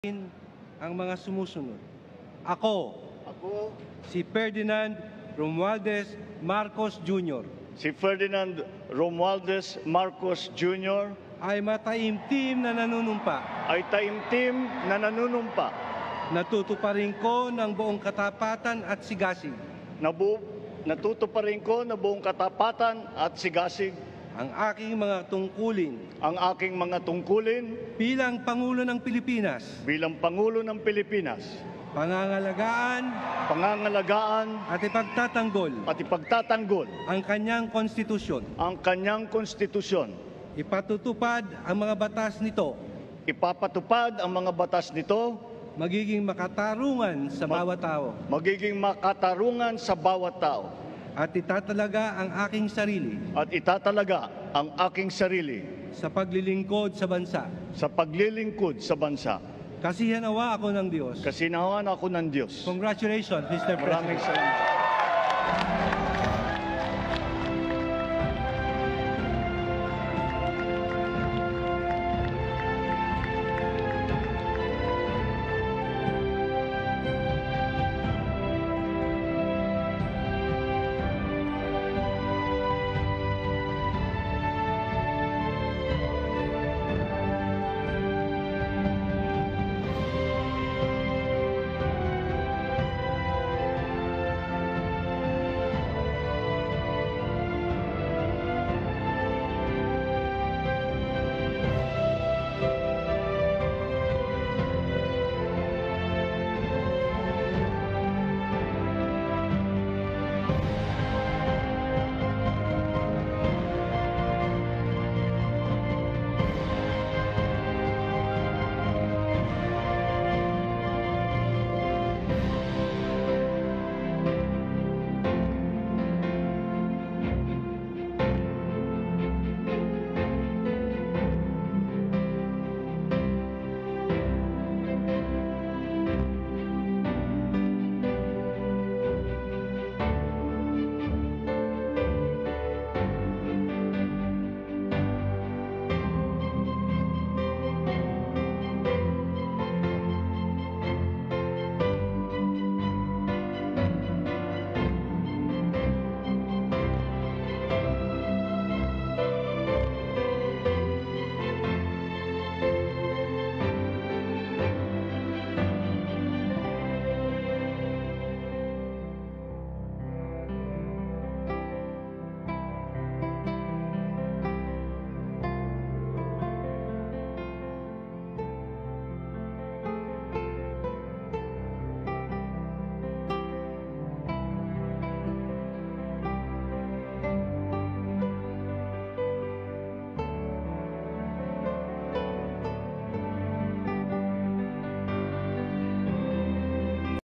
Panunumpa ni Ferdinand Romualdez ''Bongbong''Marcos jr. bilang ika 17th President ng Pilipinas
Ctto:RTVM